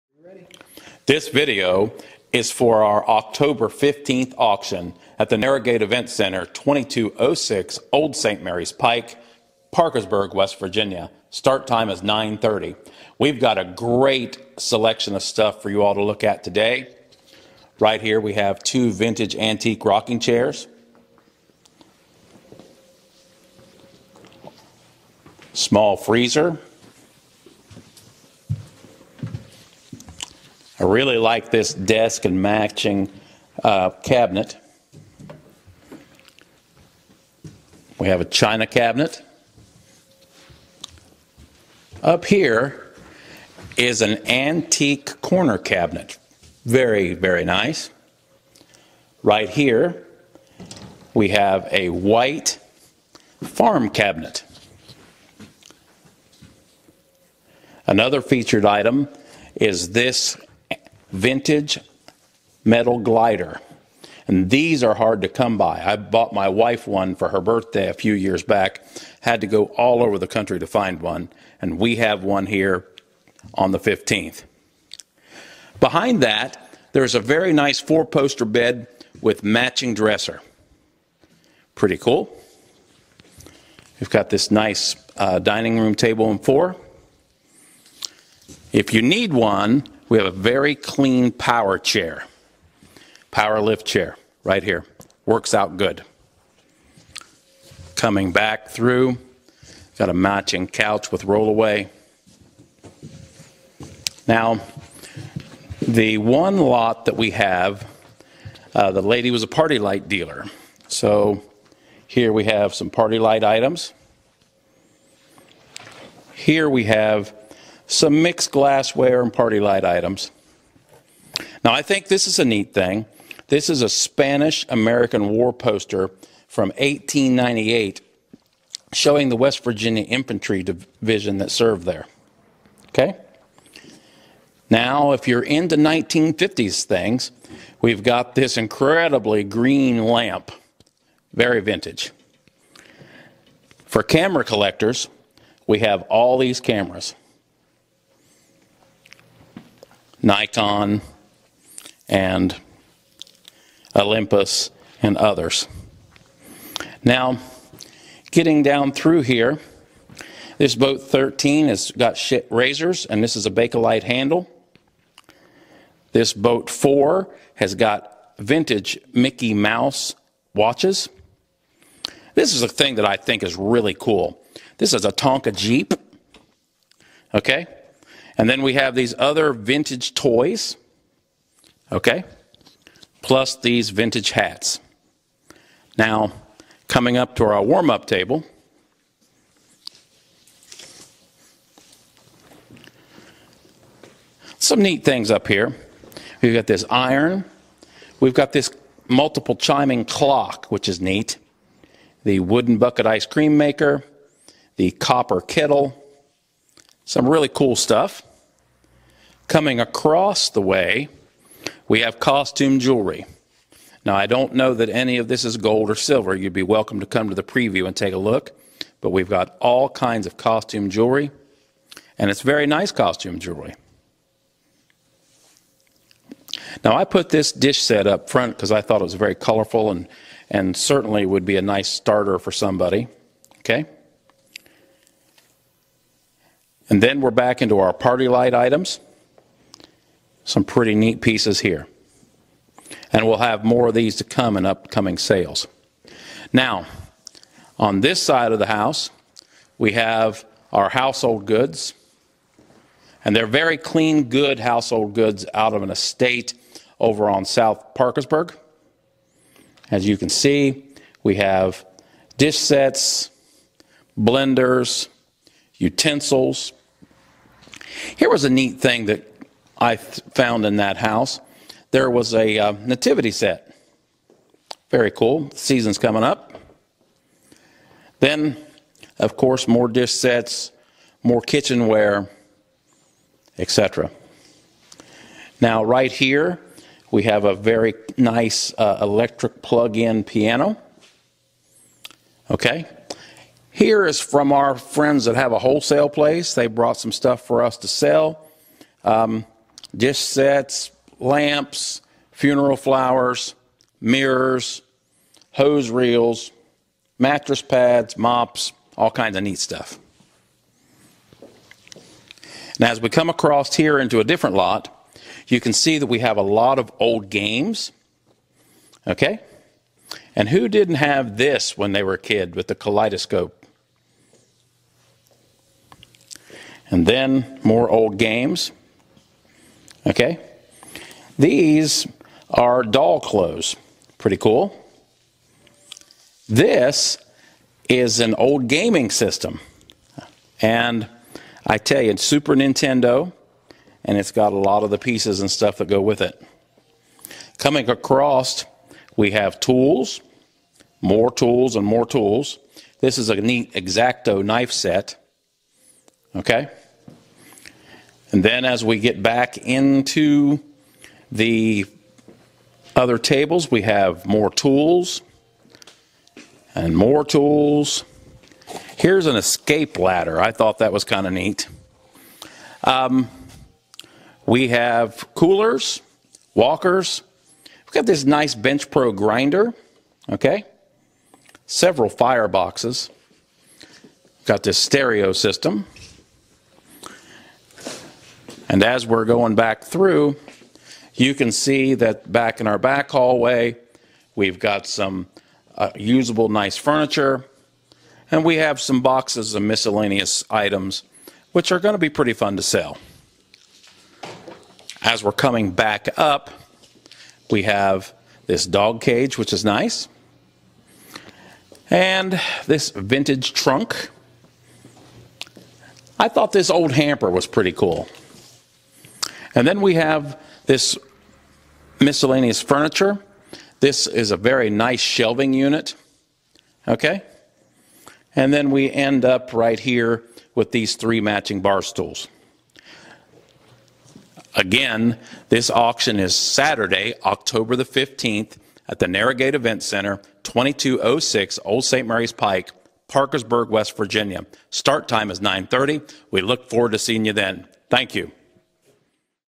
10-15-2022 Auction Preview
A preview of the items up for auction on October 15th. Start time is 9:30am and we are located at 2206 Old Saint Marys Pike Parkersburg, WV at the Narrow Gate Event Center.